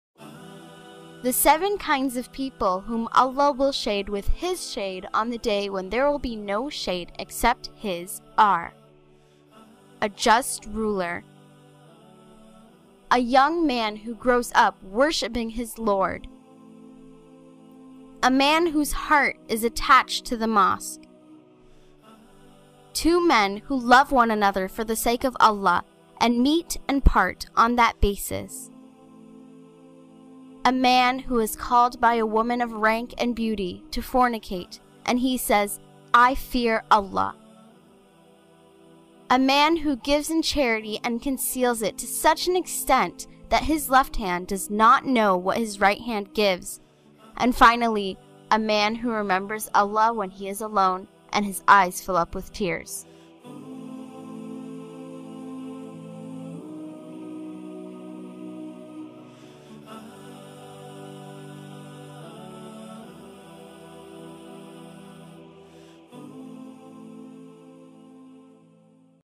7 Under The Shade Of Allah┇Hadith ┇Islamic Short Video ┇TDR Production
Video Credit to TDR Production !
7 Under The Shade Of Allah ┇Hadith ┇ Islamic Short Video ┇ TDR Production ┇
The 7 kinds of people whom Allah will Shade with his Shade on the day when there will be no Shade except His:
1. A Just Ruler.
2. A Young Man who Grows up Worshiping his Lord.
3. A Man whose Heart is Attached to the Mosque.
4. Two Men who Love One Another for the sake of Allah & Meet and Part on that basis.
5. A Man who is called by a Woman of Rank and Beauty (to fornicate) and He says 'I fear Allah.'
6. A Man who gives in Charity and Conceals it to such an extent that his left hand does not know what his right hand gives.
7. A Man who remembers Allah when he is Alone, & his eyes fill up (with tears).